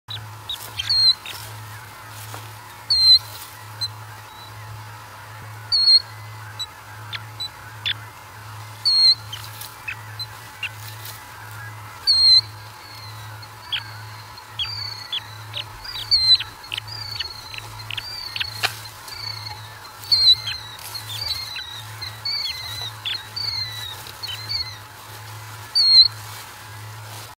New Osprey Chick Charlo Nest AND a Terrific PS, too!! 2017 06 11 09 12 53 510
How lucky am I!  Cute little brand new osprey and a grand PS from Charlotte !